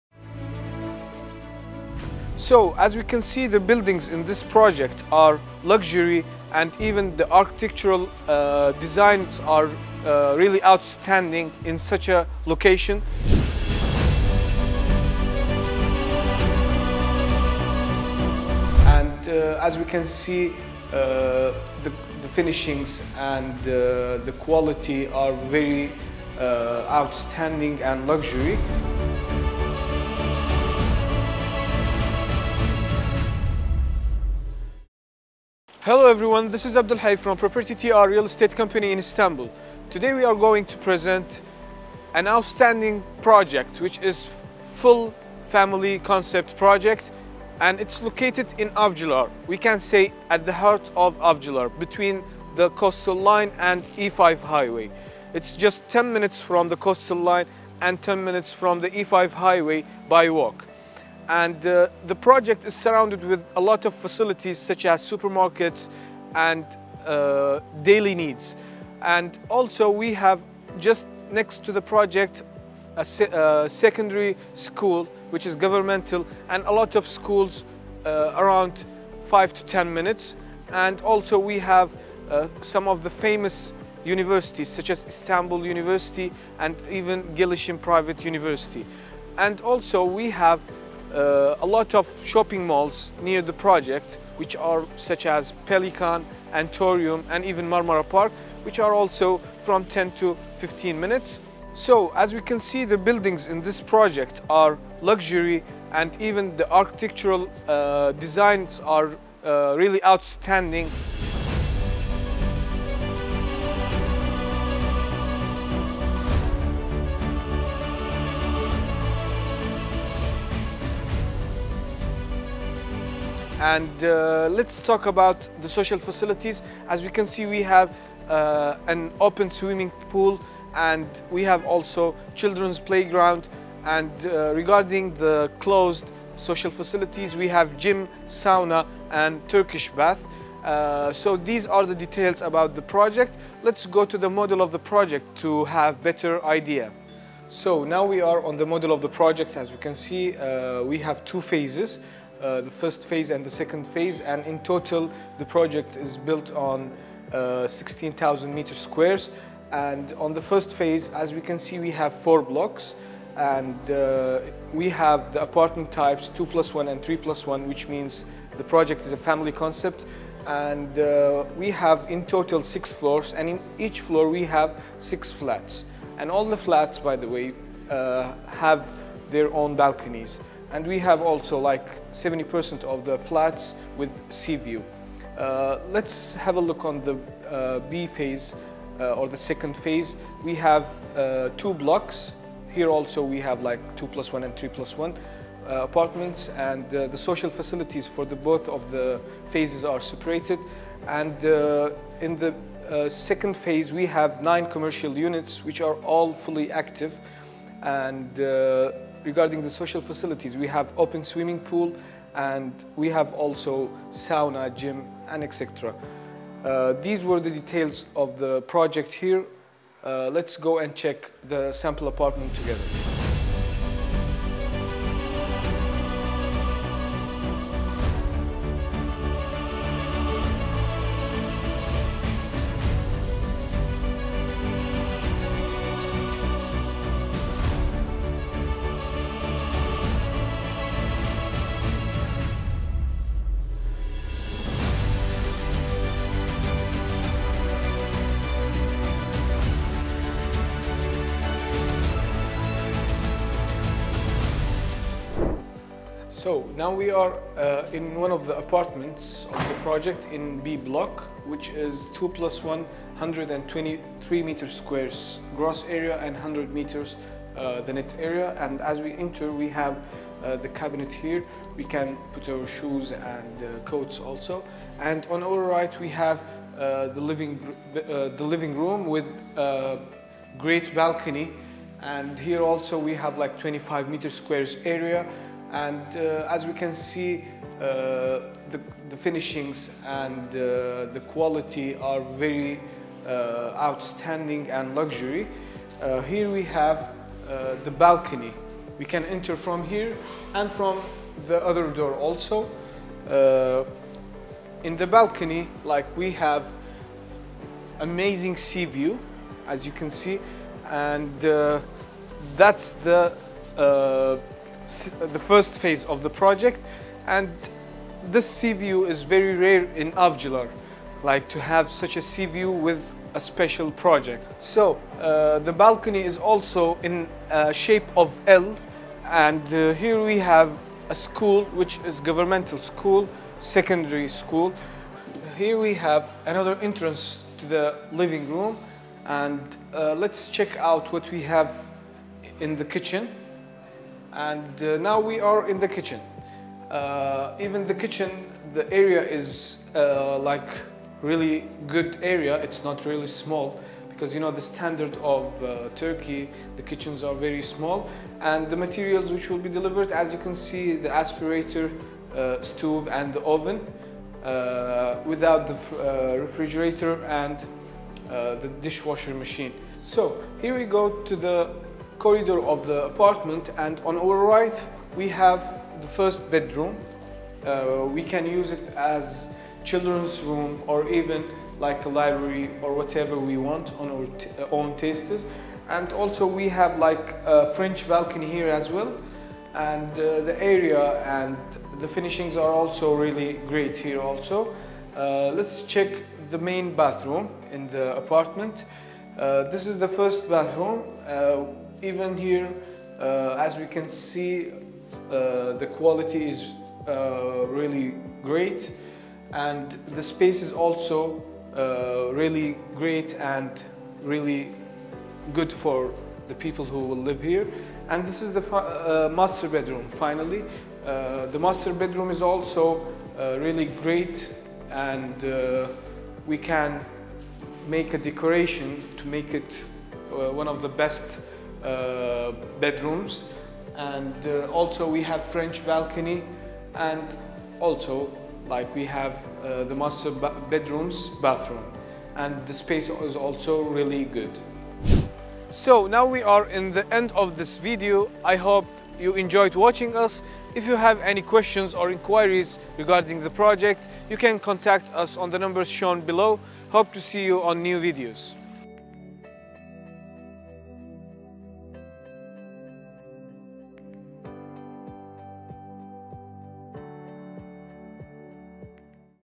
Property for Sale in Istanbul | Sea Views in Avcilar 2021
with amazing sea view in Avcilar

The project is very close to the coastline
In the Avcilar Istanbul

LOCATION:
Avcilar , istanbul , Turkey .
1 km close to Istanbul University.
1 km close to Marmara sea and lake.
near shopping malls, near Metrobus.
1 km close to private and government schools, universities, hospitals, shopping malls, hotels,

for more details about this project and the prices list